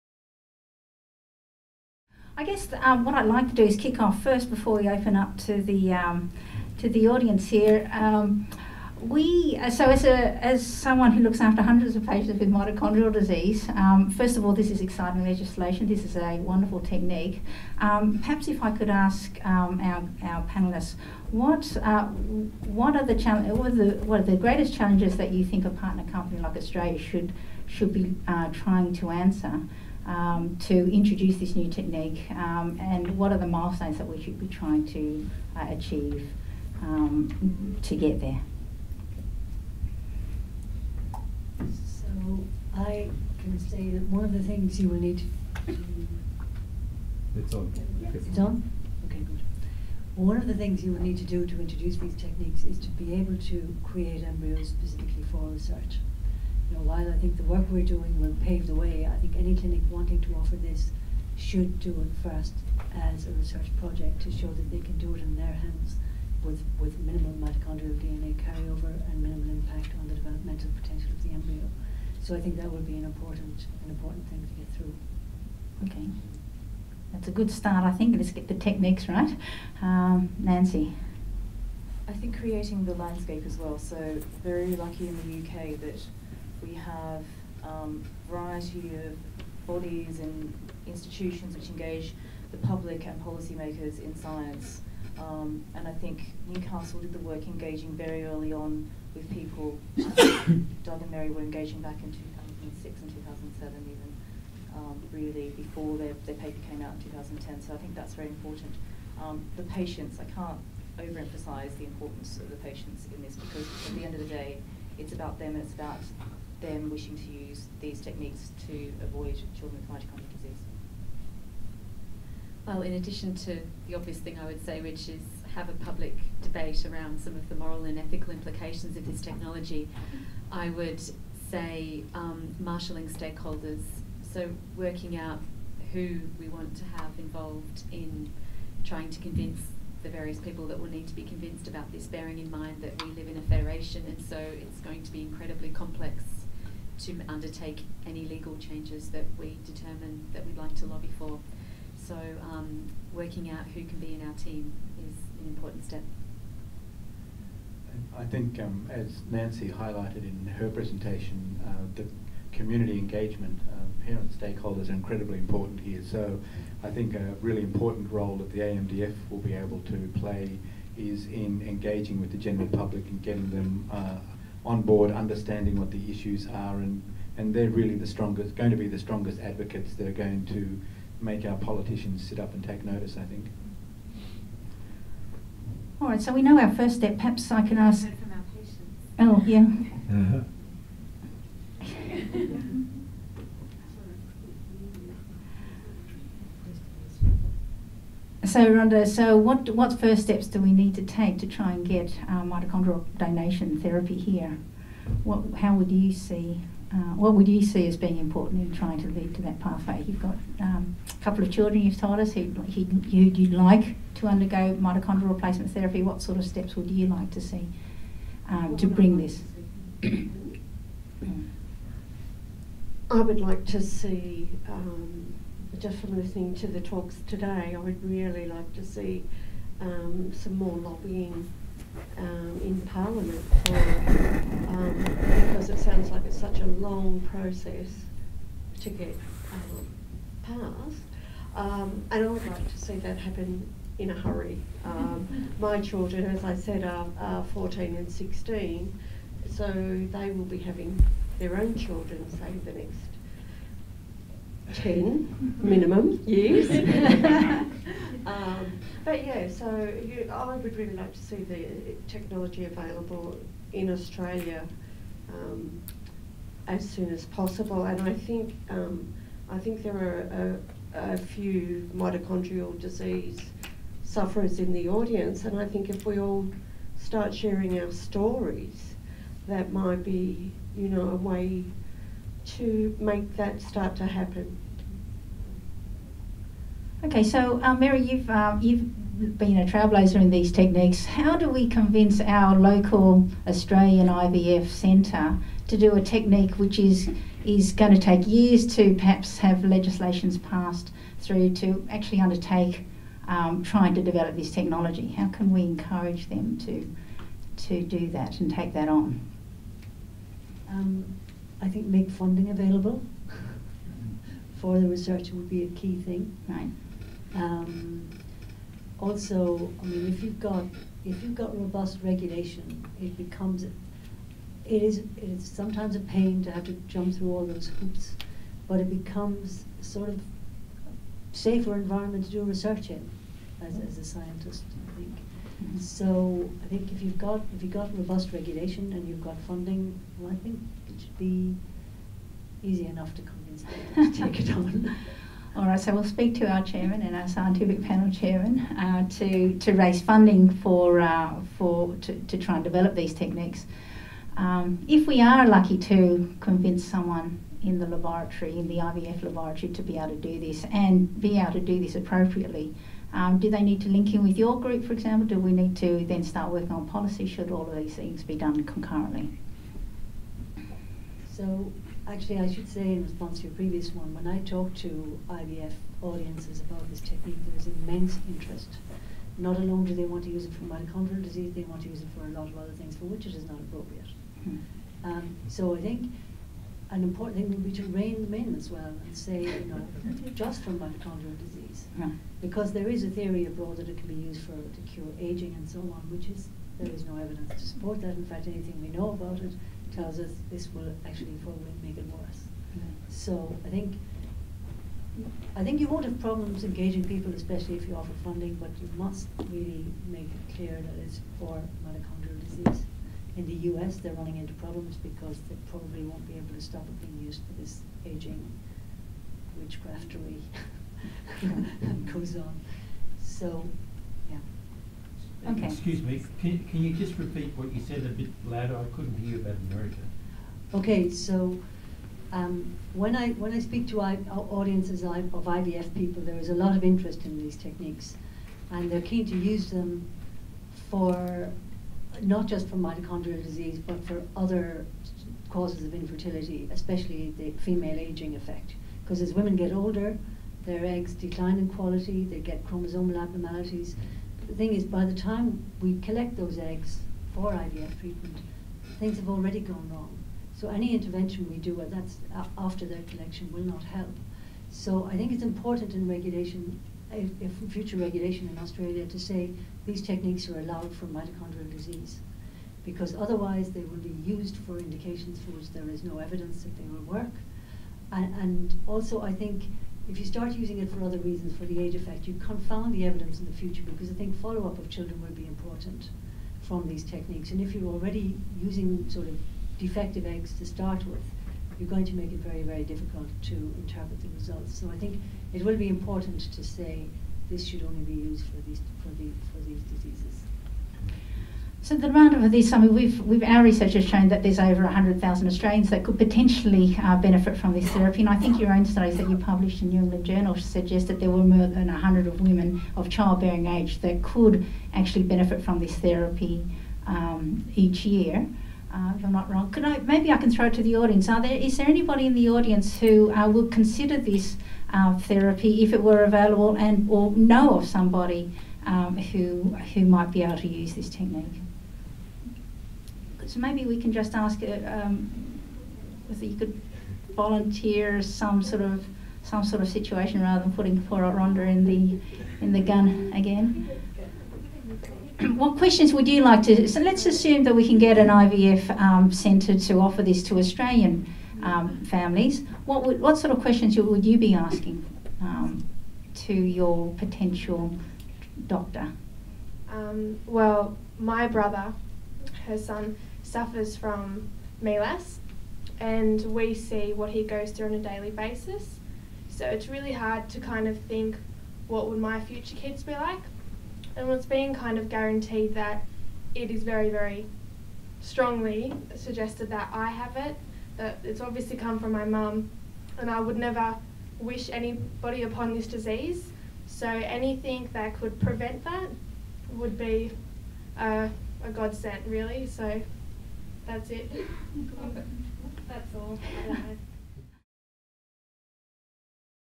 AMDF Mitochondrial Donation Symposium Sydney - Q&A Panel
Q&A Panel at the AMDF Mitochondrial Donation Symposium, 22 September 2015 at NSW Parliament House